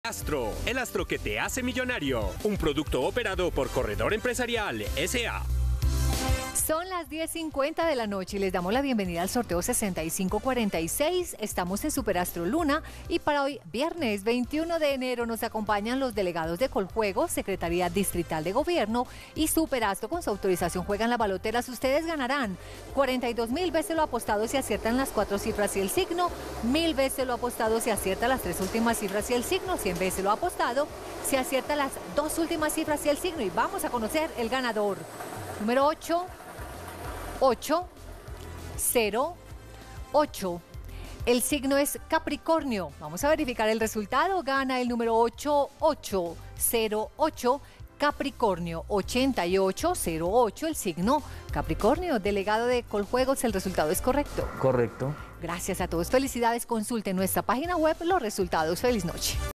ASTRO LUNA Resultado del Viernes 21 de enero de 2022 | SUPER ASTRO ✅🥇🔥💰
Hola buenas noches, te presentamos los resultados de las loterías y chances en Colombia, vamos con el Resultado para SUPER ASTRO LUNA del Viernes 21 de enero de 2022.